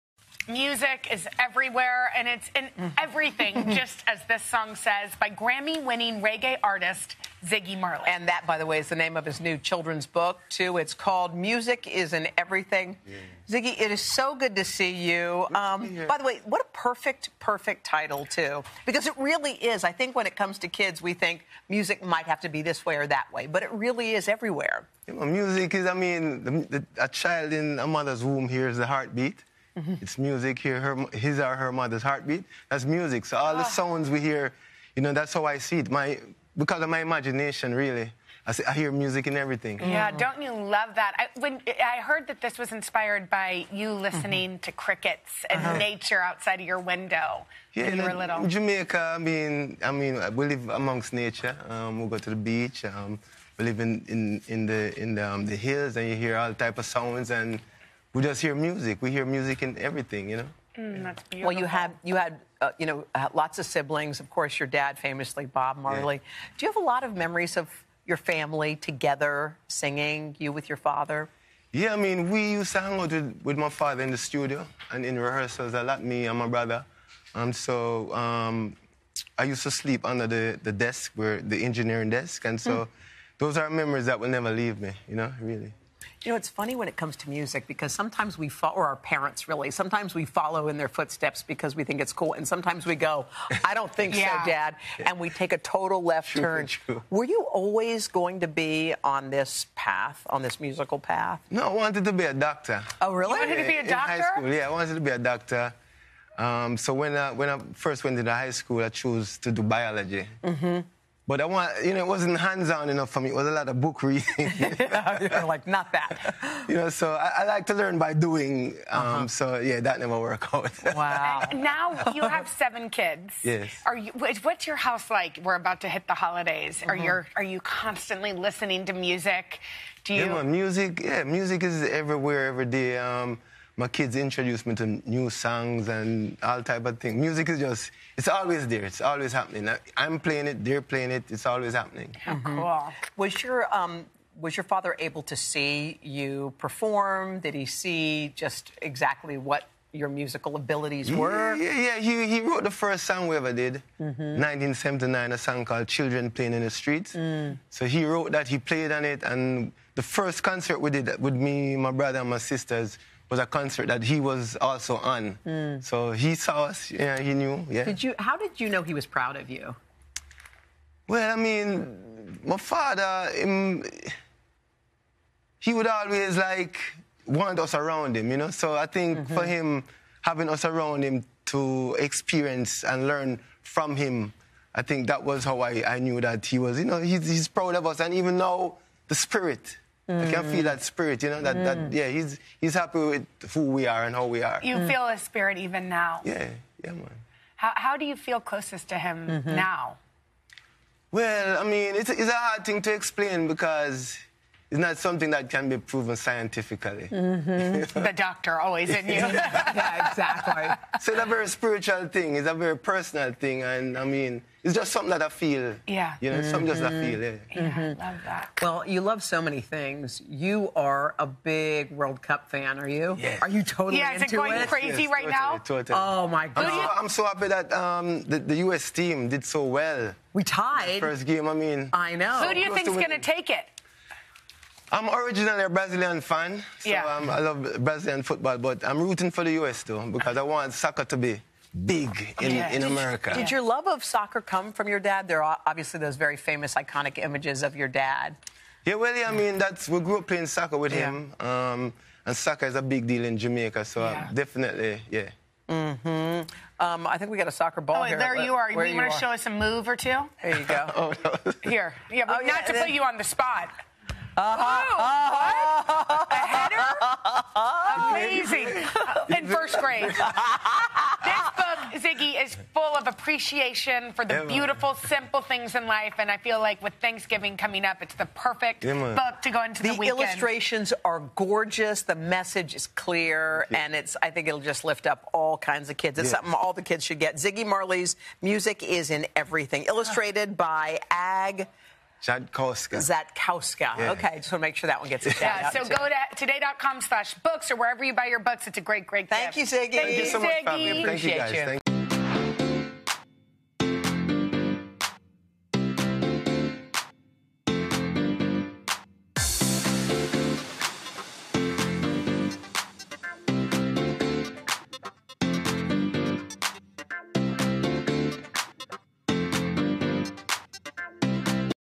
Ziggy Marley On Book ‘Music Is In Everything,’ Childhood In Jamaica
Grammy-winning reggae star Ziggy Marley talks about the inspiration behind his new book “Music is in Everything.” He also talks about what it was like growing up in a family full of performers and his passion for soccer and the World Cup.